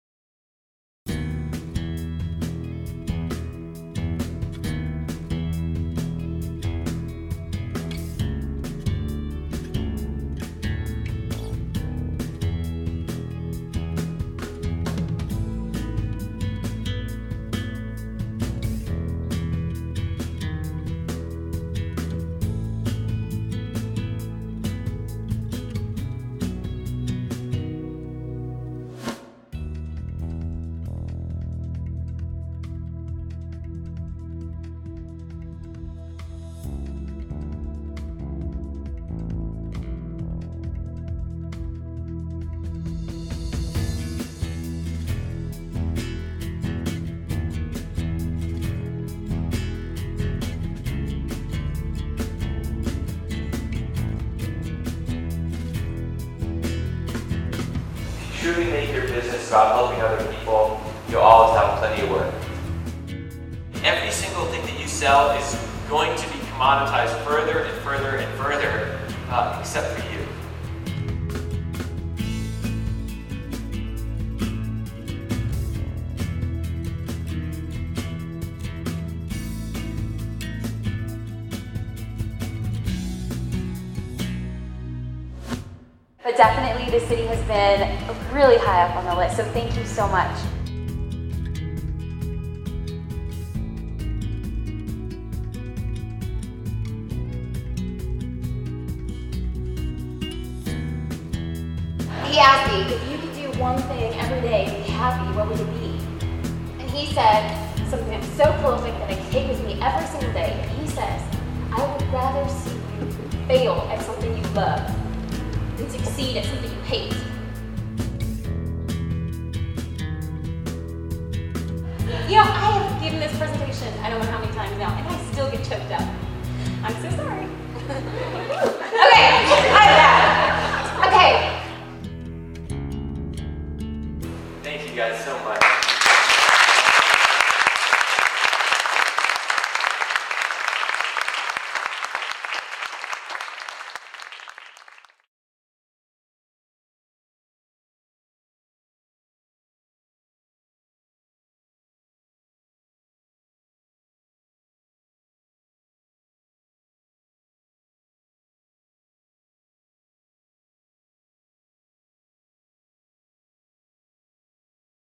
David Jay and Jasmine Star in Kansas City: Free to Succeed
David Jay and Jasmine Star were out in Kansas City a while back to generously share their knowledge and inspire fellow photographers in the Kansas City area. We had the privilege of documenting the event and put together a short promo piece for it. They are two of the kindest people and truly have a passion for what they do and empowering others to do the same. It was a great time and it was great to be able to meet and catch up with other photographers in the Kansas City area.

It is my personal ambition to never stop learning and the topics and subject matter that DJ and Jasmine touched on are universal and inspirational to anyone who listens. Even though I was technically working during the event, I still got a lot out of it and was glad that I was fortunate enough to be a part of it.

Thank you, DJ and Jasmine for sharing your hearts, passion, and experiences! Definitely gave us a lot to think about!